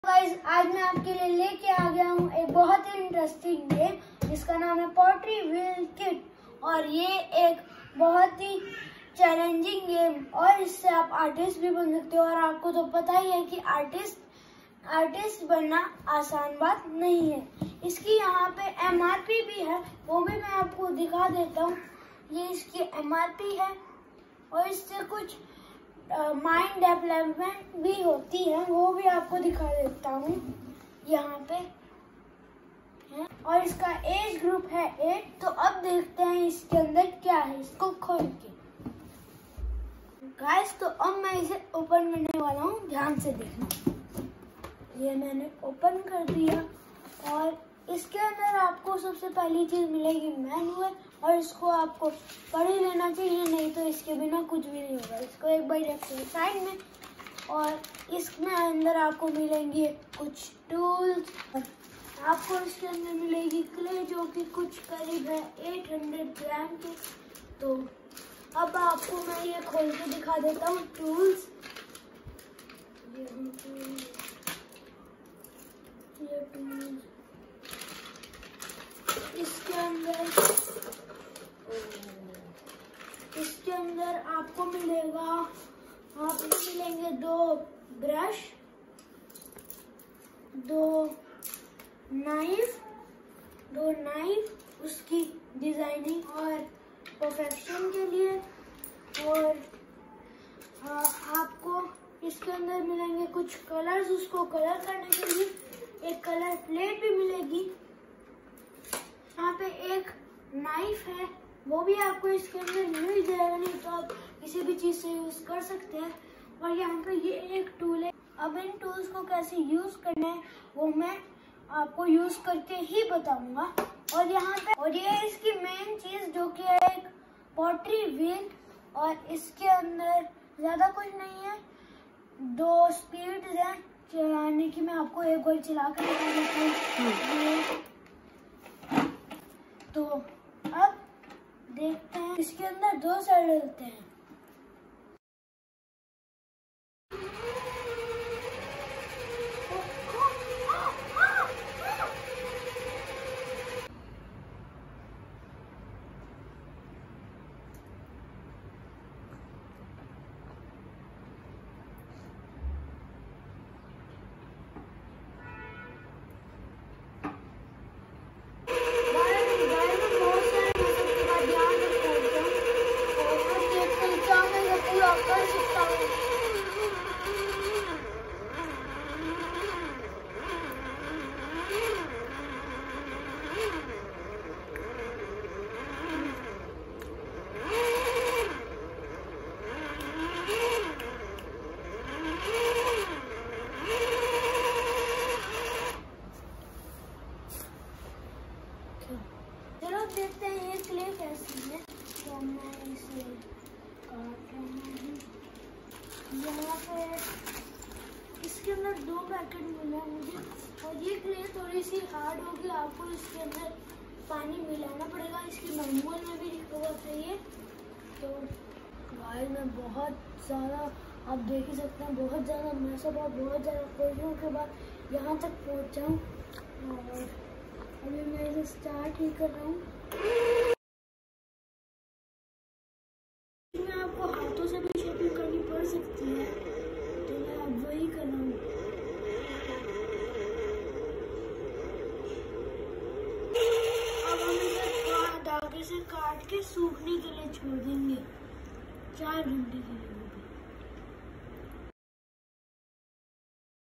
Pottery wheel kit review
Review of pottery wheel electronic kit
Company : annie
Price -600 to 700
Age group- 8+
Level: Medium to hard
Needs patience and talented hand